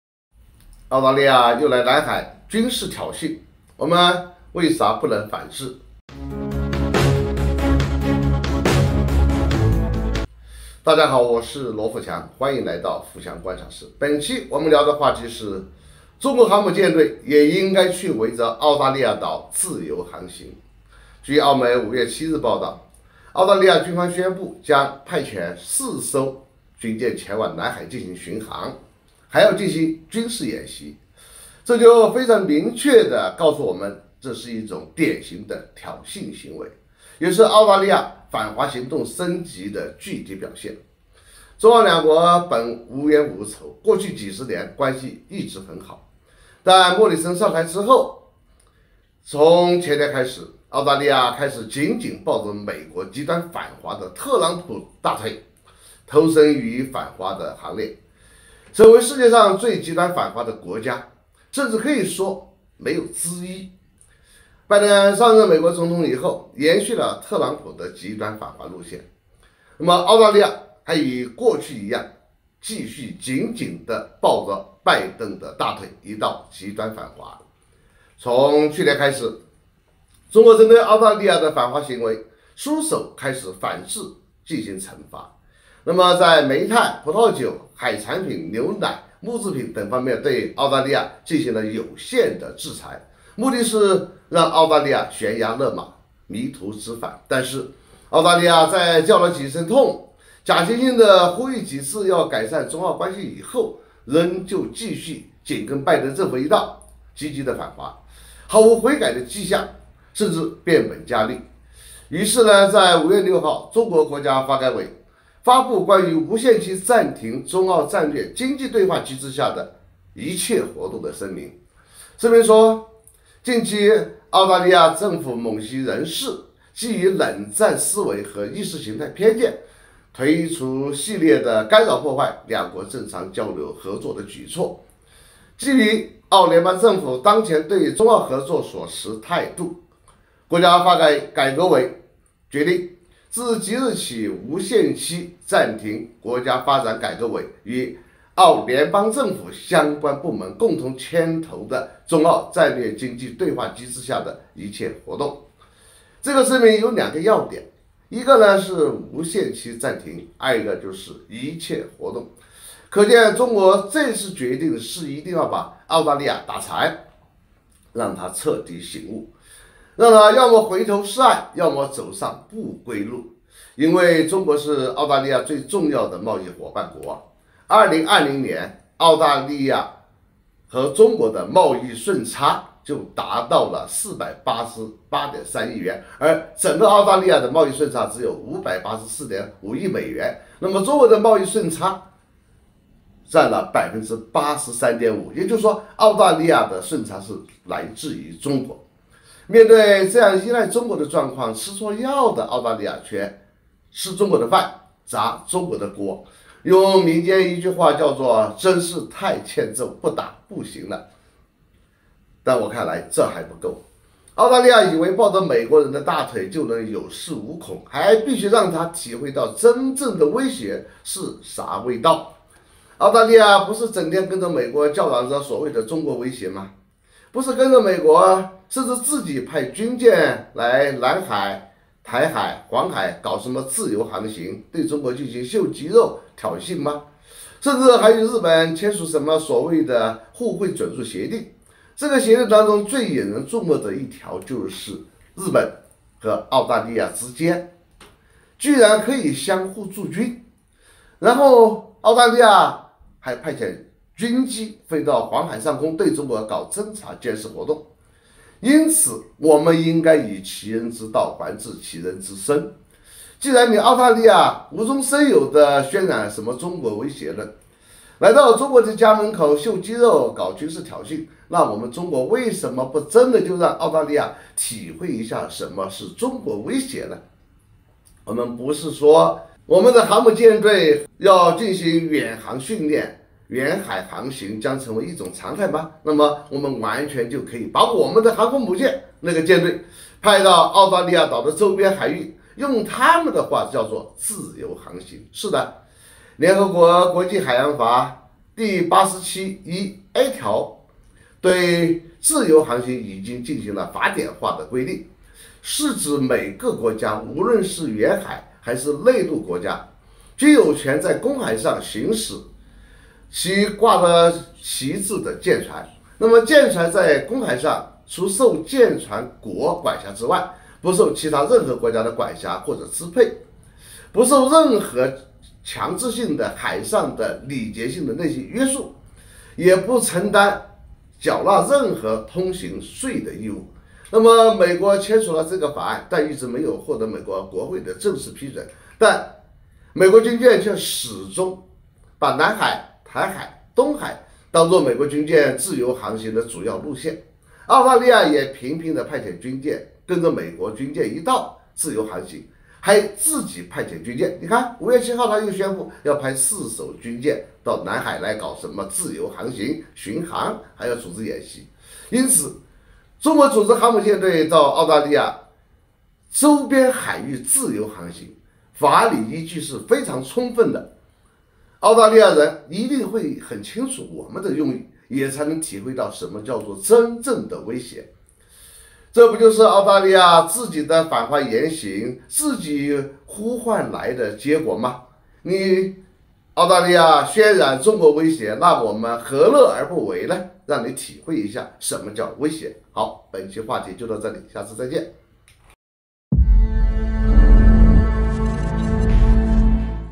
不但沒服軟，澳大利亞還派4艘軍艦來南海挑釁，中國為何不反制？【罗富强】
澳大利亞又來南海軍事挑釁，我們為啥不能反制
大家好，我是羅富強，歡迎來到富強觀察室
本期我們聊的話題是，中國航母艦隊也應該去圍著，澳大利亞島自由航行
不但沒服軟，澳大利亞還派4艘軍艦來南海挑釁，中國為何不反制？
強國時頻 軍事 政治 我們的影片會遵循公平、尊重的原則，確保不會包含任何仇恨言論的元素。